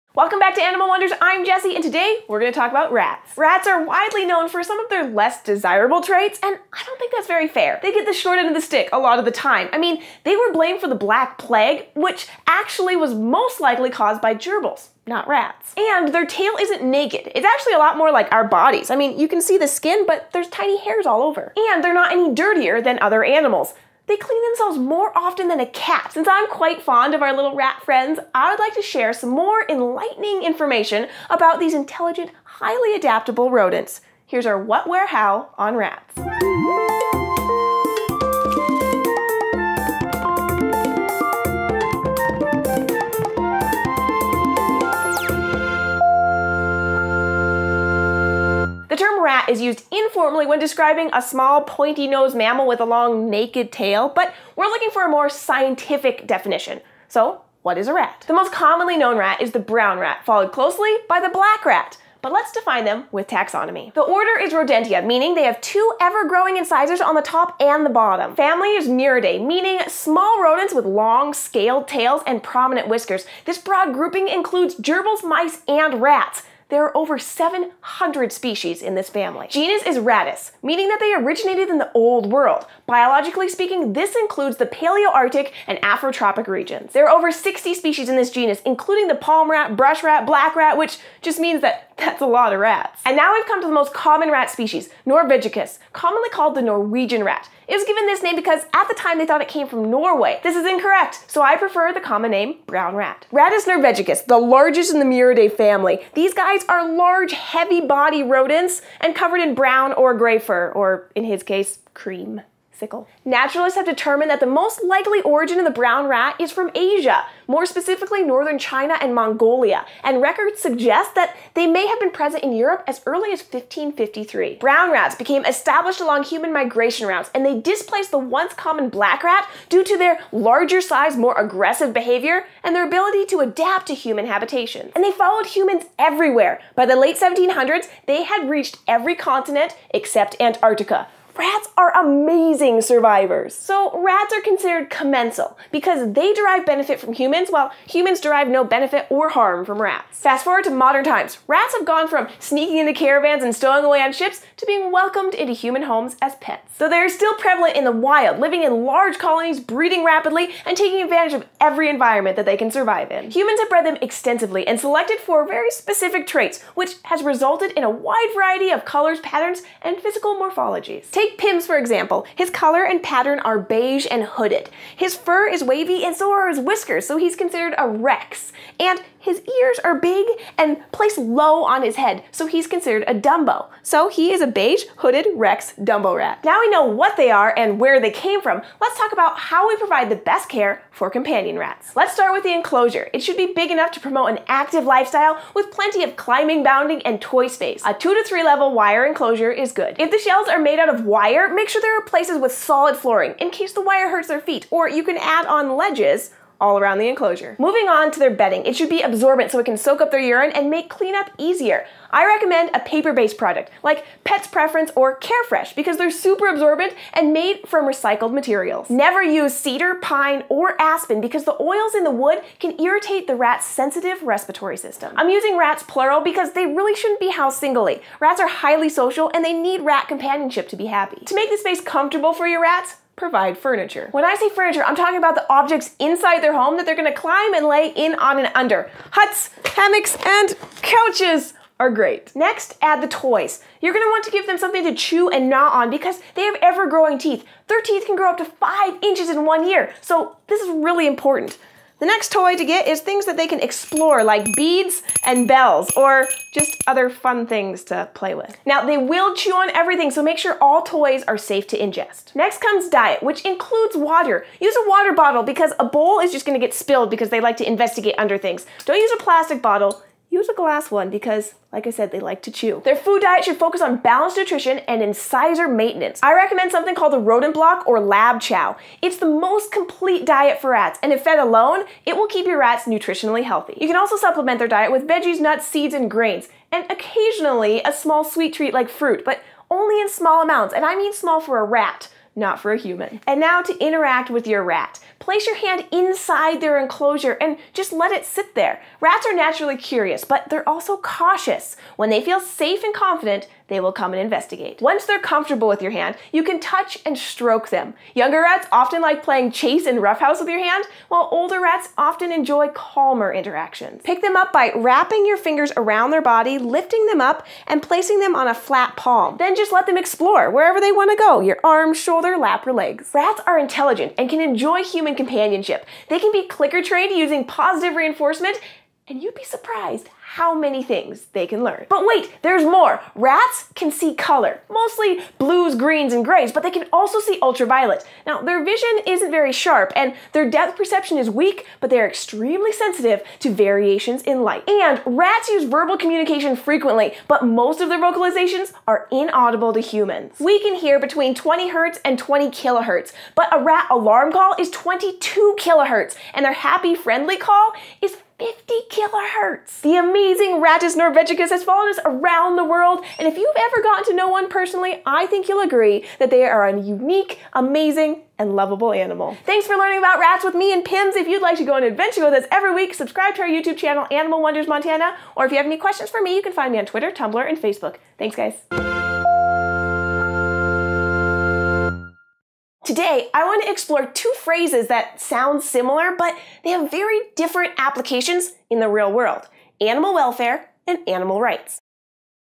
Rats! What, Where, and How
Jessi explains what rats are, where they came from, and how to care for them in captivity. Rats are awesome in so many ways!

Robert K.
Erienne Keleher
Jacynthe Viau
Brandon Metheny
Gummi Kr.
Jacob Lockey
Richard Campbell

Amazon Wishlist